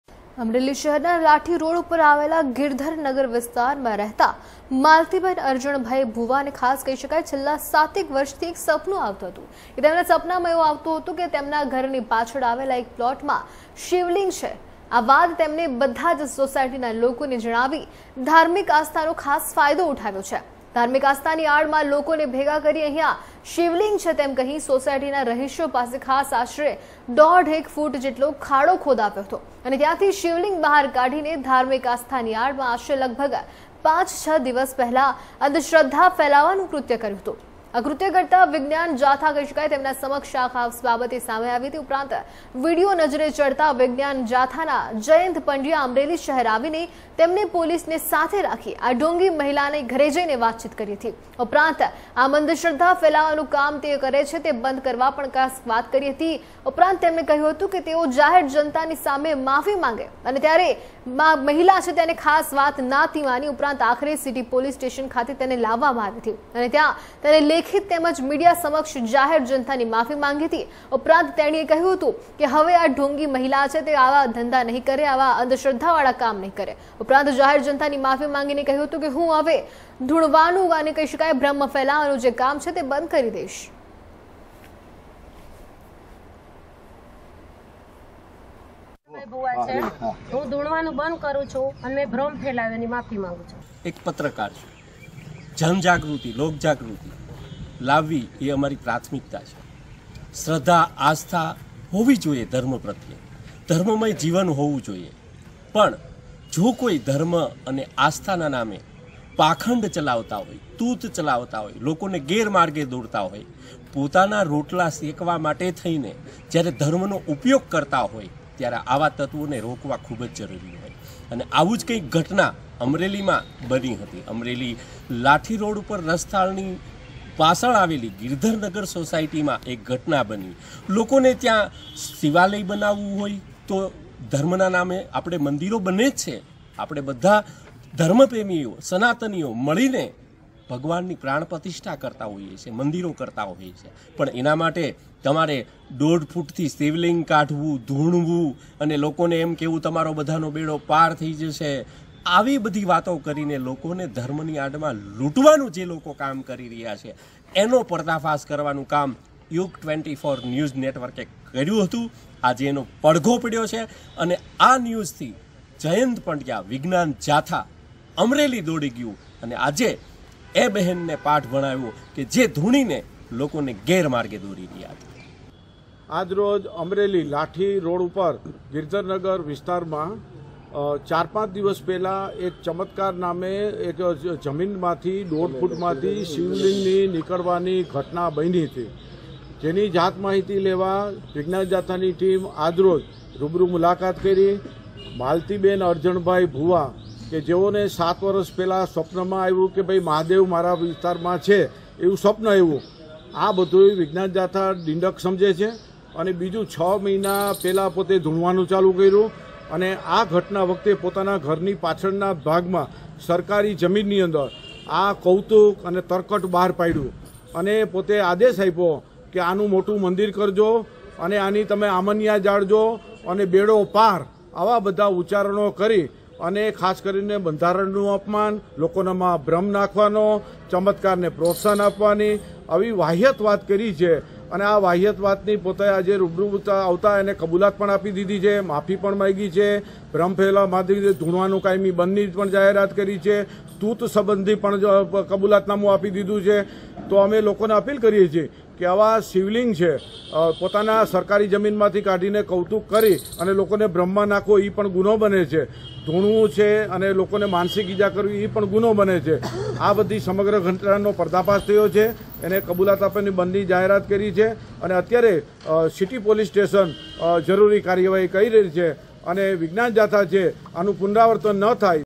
અમરેલીમાં આસ્થાની આડમાં ફેલાવાઈ અંધશ્રદ્ધા - India News Gujarat
અમરેલી શહેરના લાઠી રોડ ઉપર આવેલ ગીરધર નગર વિસ્તારમાં રહેતા માલતીબેન અરજણભાઈ ભુવાને છેલ્લા સાતેક વર્ષથી સપનું આવતું કે પોતાના ઘરની પાછળ આવેલ એક પ્લોટમાં શિવલિંગ છે. આ વાત બધા સોસાયટીના લોકોને જણાવી ધાર્મિક આસ્થાનો ફાયદો ઉઠાવી ધાર્મિક આસ્થાની આડમાં લોકોને ભેગા કરીને અહીંયા શિવલિંગ છે તેમ કહી સોસાયટીના રહીશો પાસે આશરે દોઢ એક ફૂટ ખાડો ખોદાવી ત્યાંથી શિવલિંગ બહાર કાઢીને ધાર્મિક આસ્થાની આડમાં આશરે પાંચ છ દિવસ પહેલા અંધશ્રદ્ધા ફેલાવવાનું કૃત્ય કર્યું હતું.

Maltiben Arjanbhai Bhuva, who lives in Girdhar Nagar area on Lathi Road in Amreli city, has been dreaming for the past seven years that there is a Shivling in a plot behind his house. By telling this to all the people of the society, they took advantage of the religious faith and gathered the people under the guise of religious faith, saying that there is a Shivling here, dug a pit about one and a half feet near the residents of the society and took out the Shivling from there, and spread superstition under the guise of religious faith about five or six days ago.